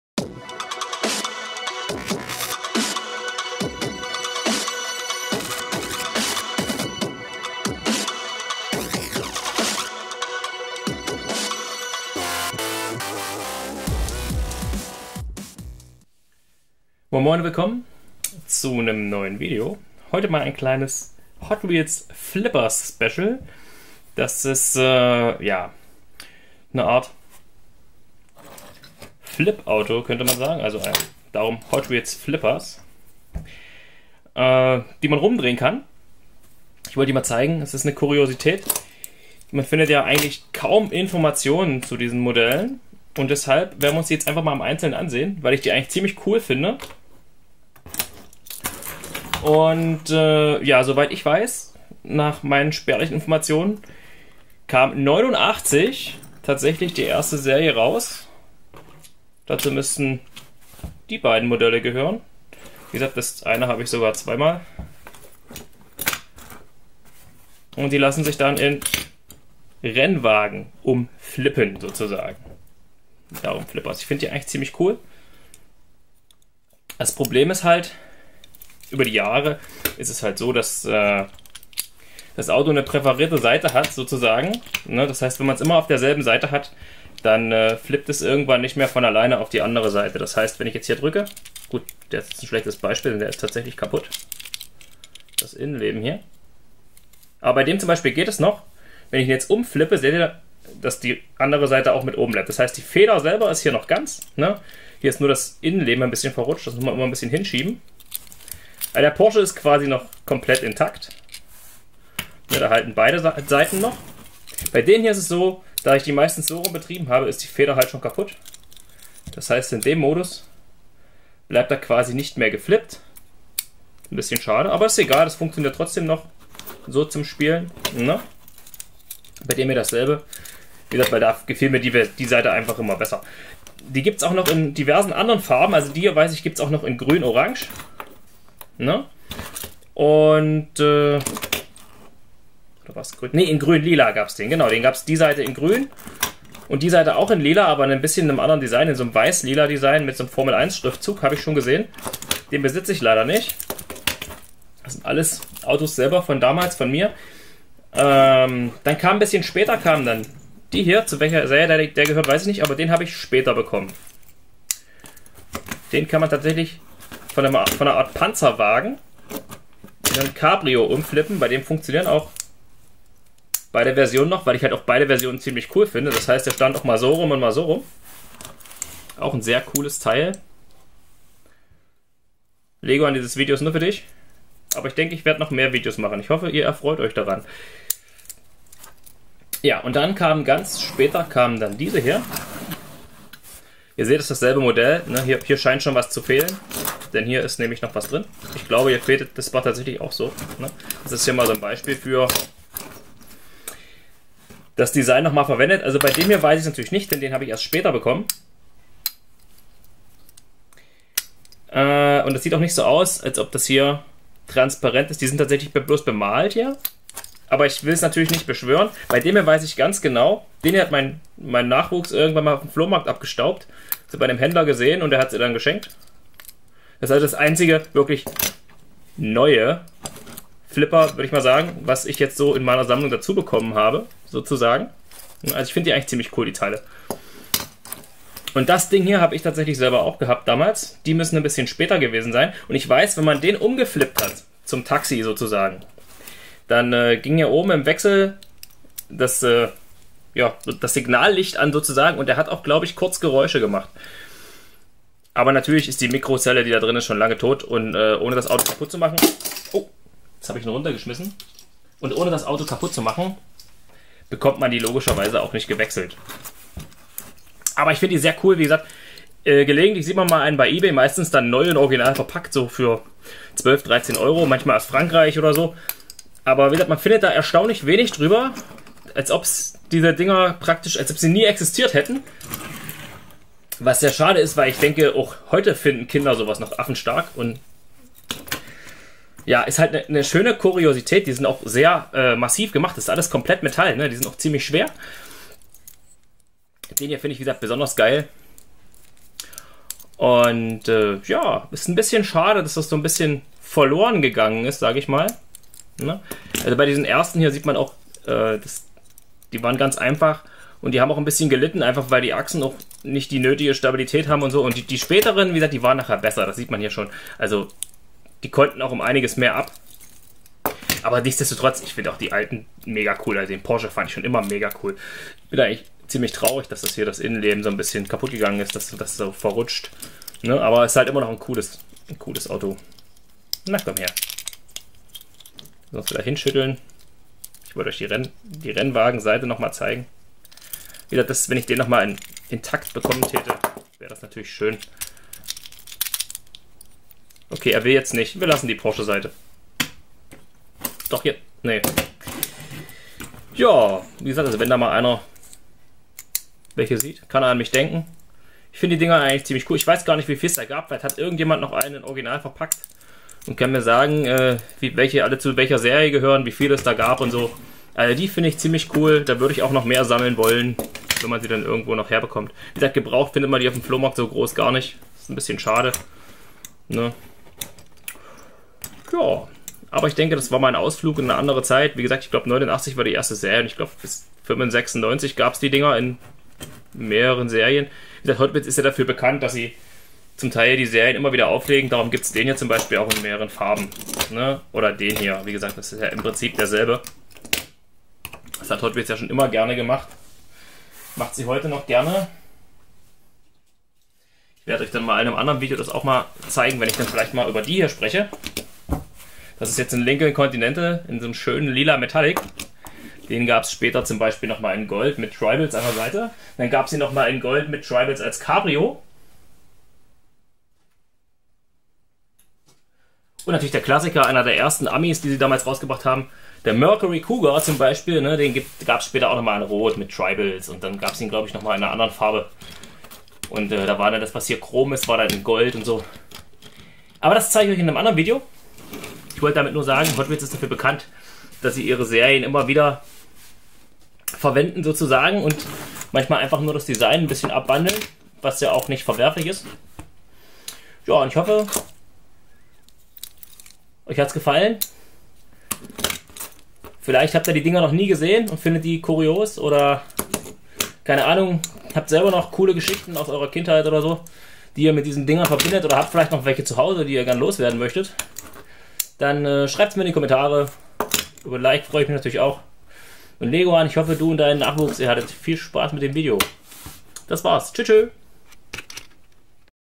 Hotwheels Flippers Special
Mal ein kleines Special über diese kleine Kuriosität aus der Hotwheels Palette, die leider schon lange Geschichte ist.
Viel Spaß